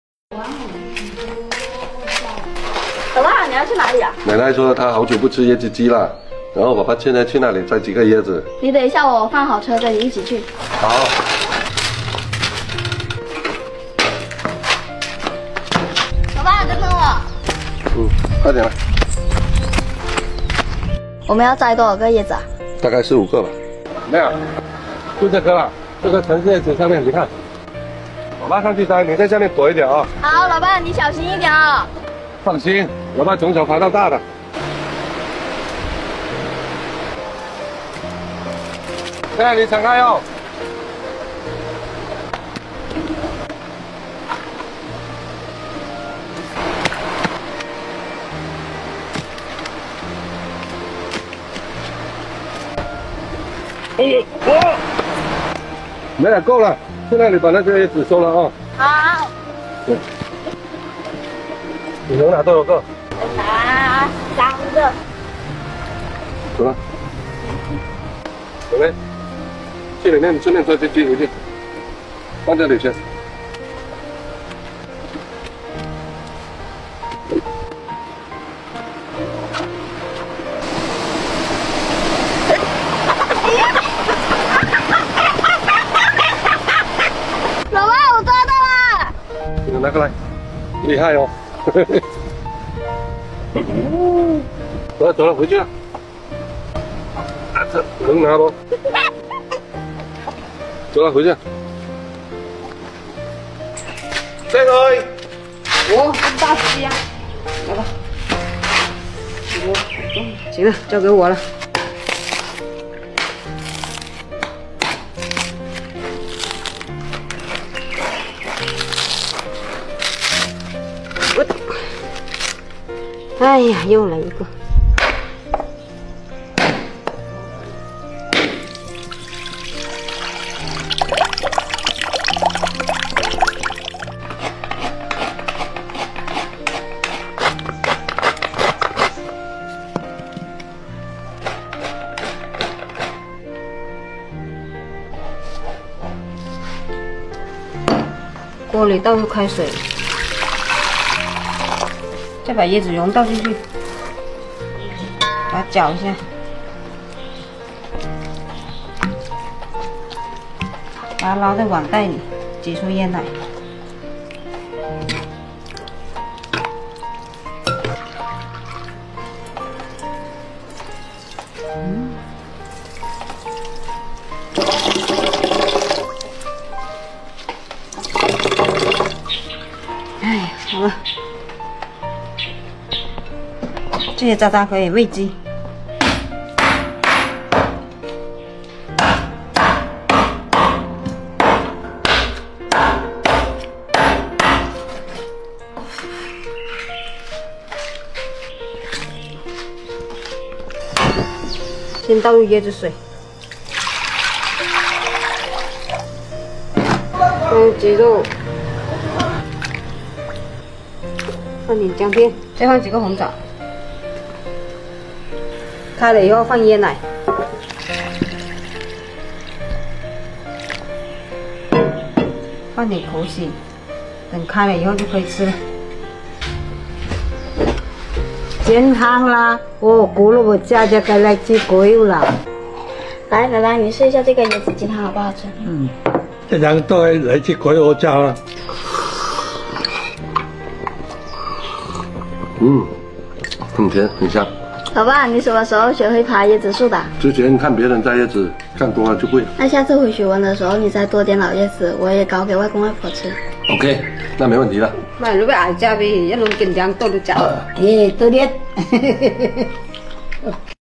降温了，奶奶想吃椰子鸡，父女立马出手，温润美味的椰子鸡汤，温暖幸福的三代同堂。
动物总动员,美食,乡村,农村,鸡汤,乡村生活,想吃,农村生活,农村美食,出海捕鱼,动物总动员-春日萌宠随手拍